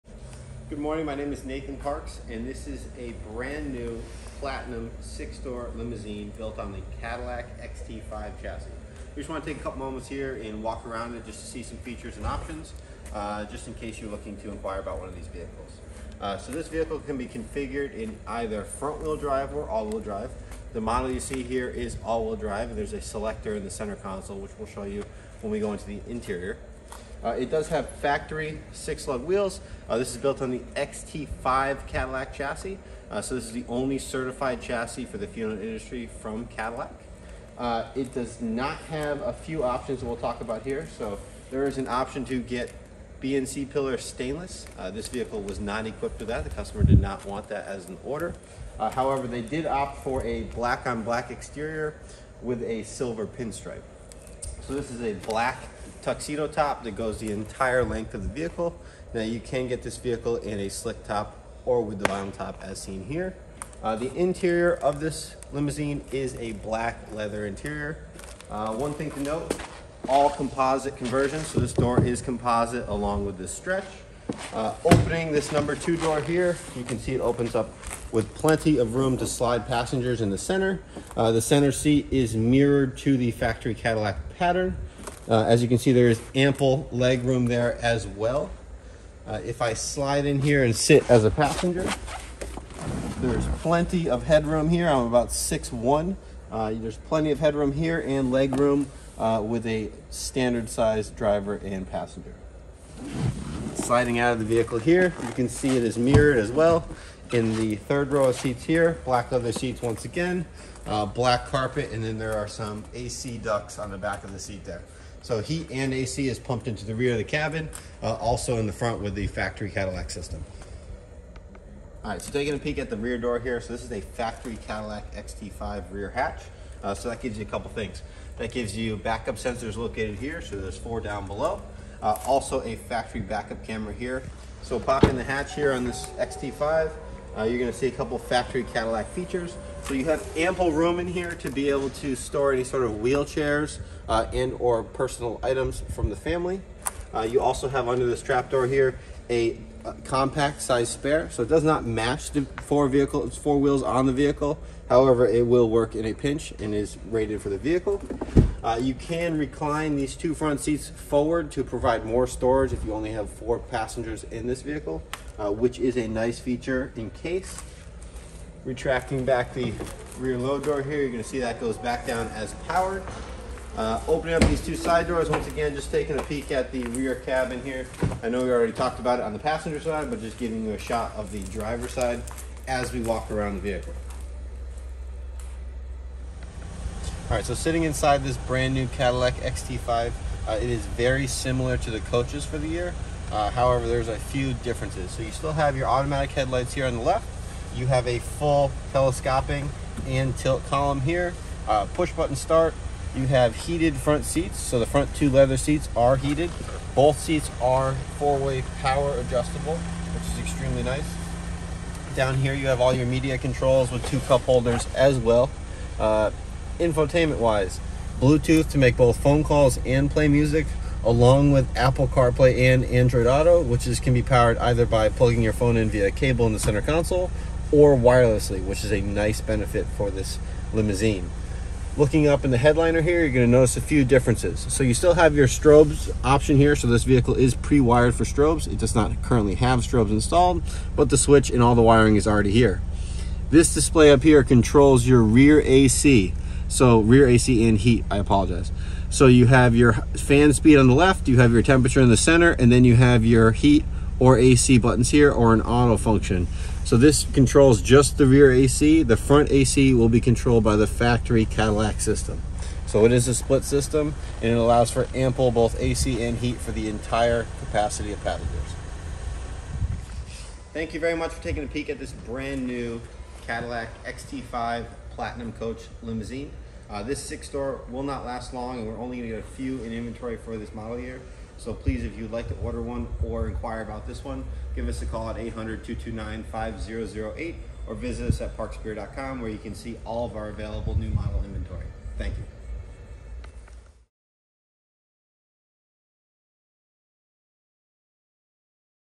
2022 Platinum Coach Cadillac XT5 48" 6-Door Limousine- DEMO (NZ168442)
Take a look around the all new Platinum Coach Cadillac XT5,  48" 6-Door Limousine. This limo will certainly attract funeral directors. Features,Android Auto, Apple Car Play, Back Up Camera, Back-Up Sensor, CUE Information and Media Control System, Compact Spare Tire, Full Vinyl Top, Heated Seats, Heated Steering Wheel, Keyless Start (Push Button), Lane Assist, Pre-Wire for Strobes, Tinted Windows & more.